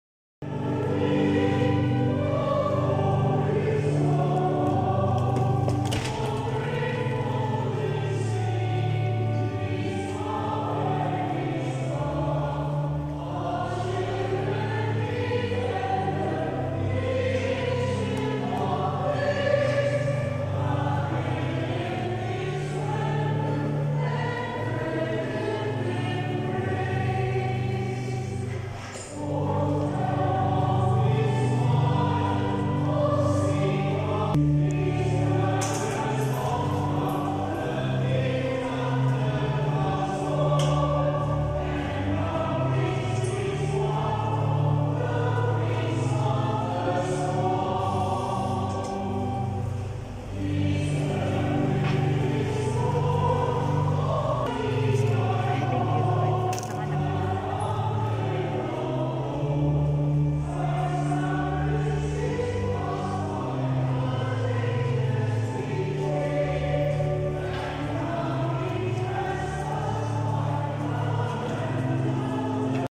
Palm Sunday Blessings/ /Catholicholymass
Jesus entering into Jerusalem and being greeted by the people waving palm branches, for Christians, Palm Sunday it is a reminder of the Welcoming of Jesus into our Hearts, and willingness to Follow Him 🙏🙏